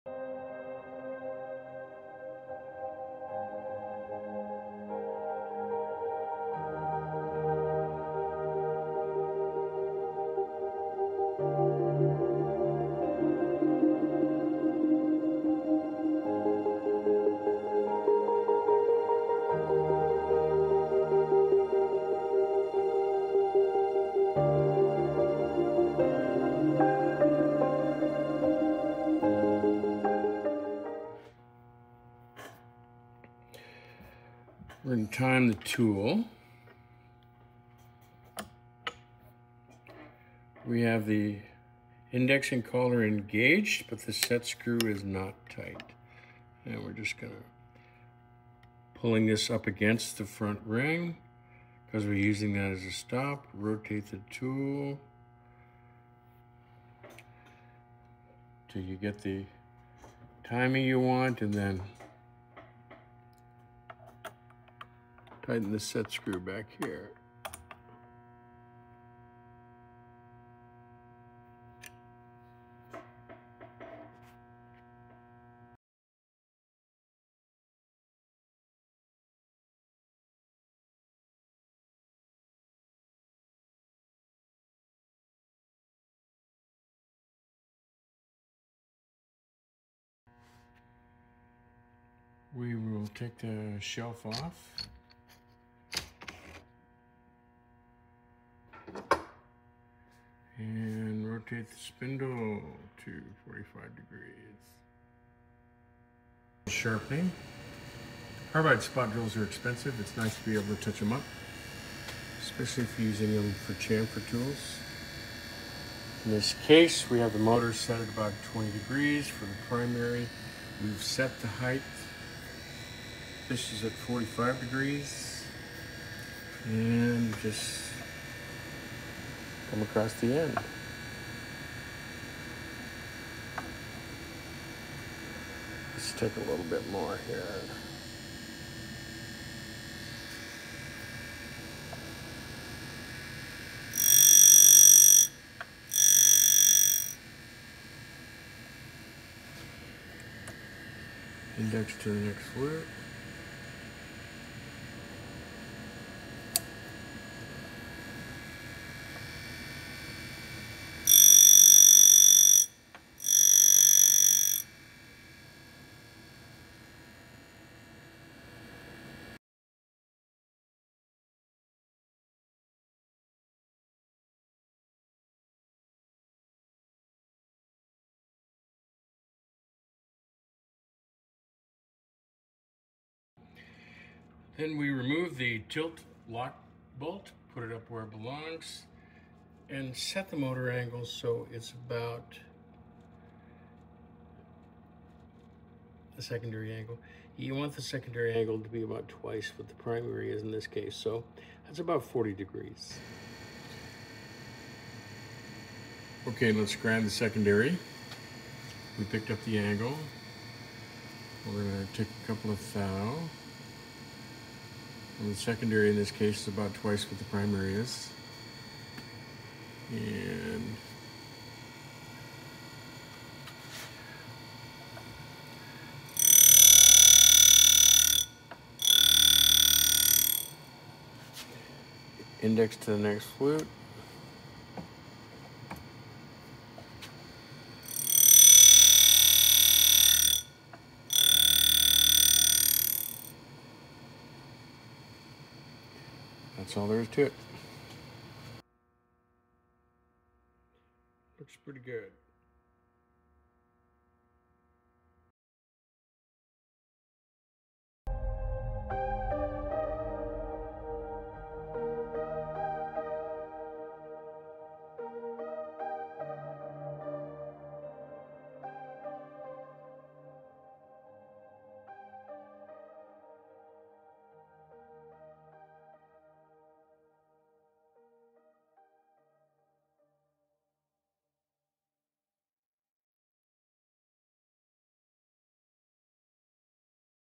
How to Sharpen a Carbide spot drill
Cuttermaster  Tool Grinder sharpening a carbide spot drill,  (easy)
This can be done on a CM01 , JXT , or a Cuttermaster Professional CM01P